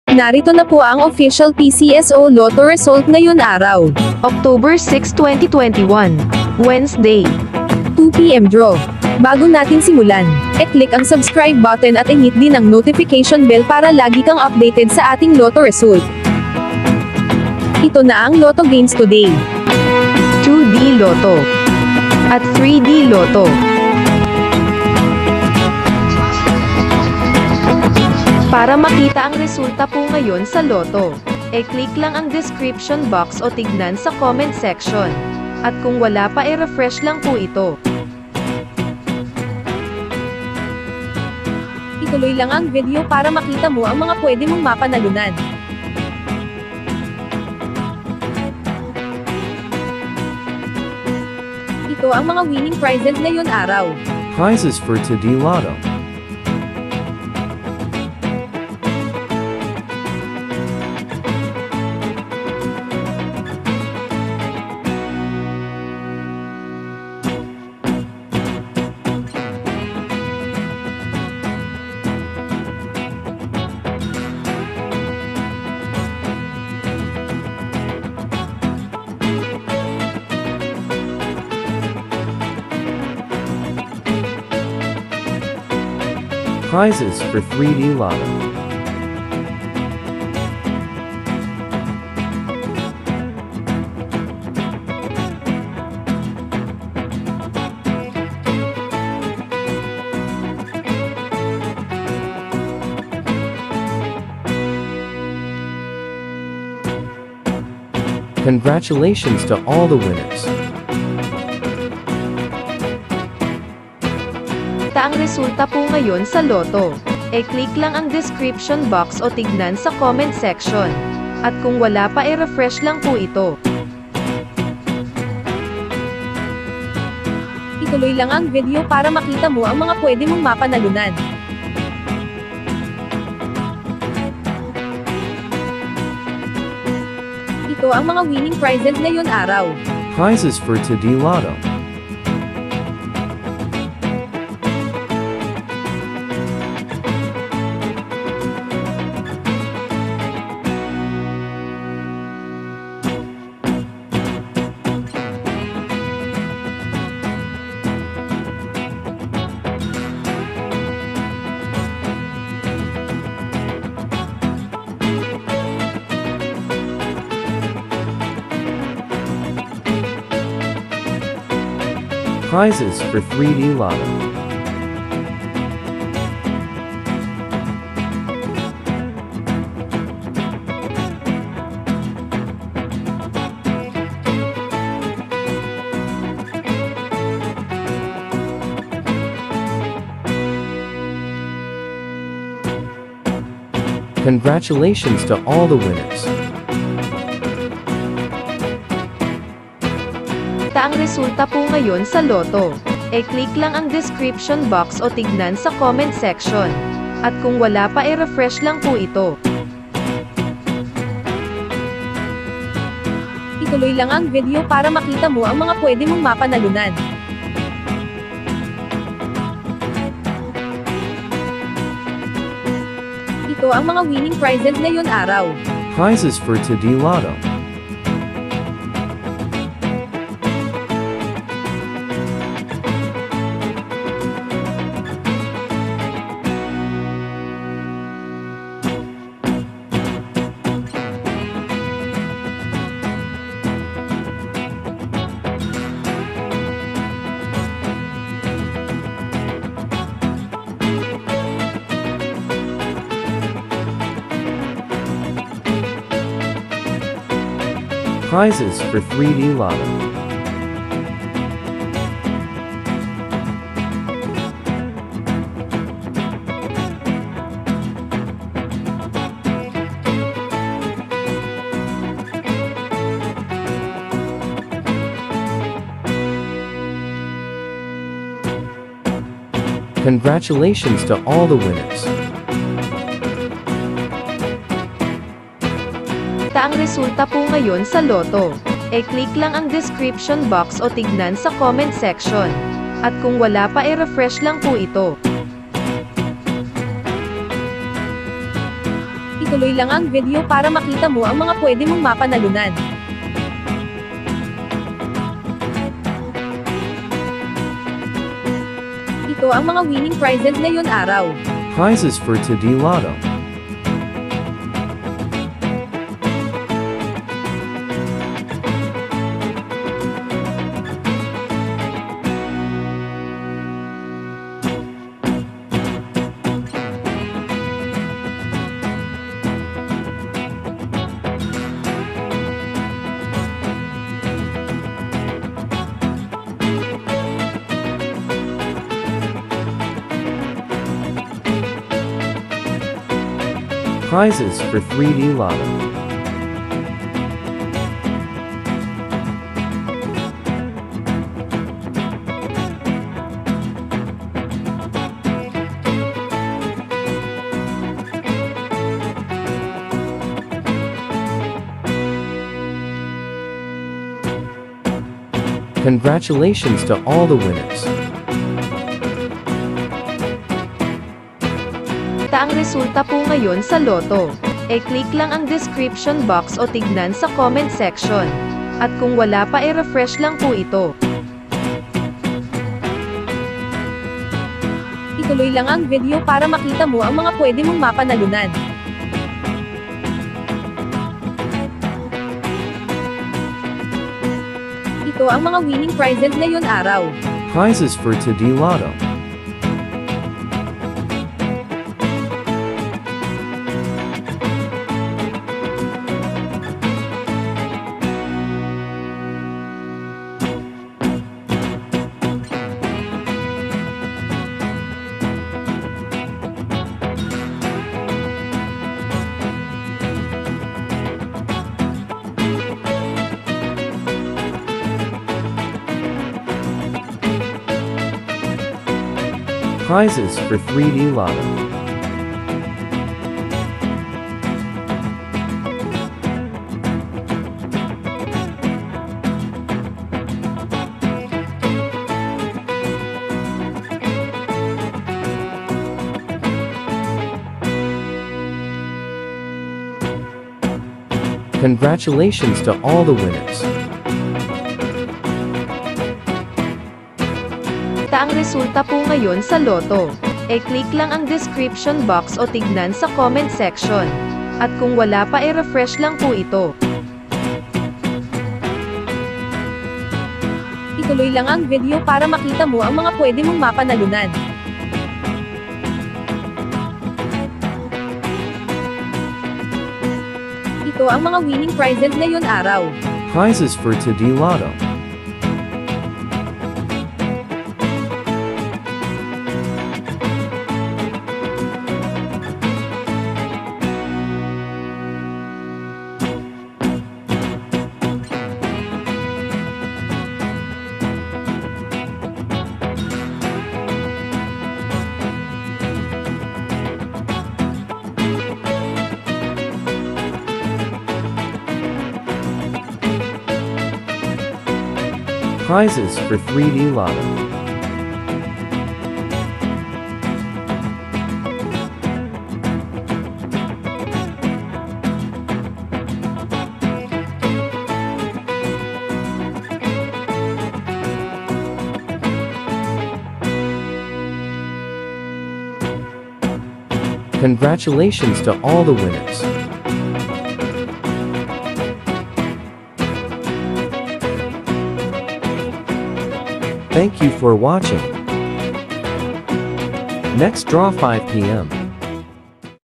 LOTTO RESULT TODAY 2PM DRAW - OCTOBER 06, 2021 | 3D | 2D | SWERTRES | EZ2
ITO NA PO ANG RESULT NGAYON HAPON SA LOTTO 2PM DRAW
LOTTO RESULT TODAY – OCTOBER 06, 2021

2D LOTTO→ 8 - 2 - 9
3D LOTTO→ 03 - 22

SUPPORT ME... SUPPORT ME... SUPPORT ME... ❣️for donating.❣️ Any amount will accepted.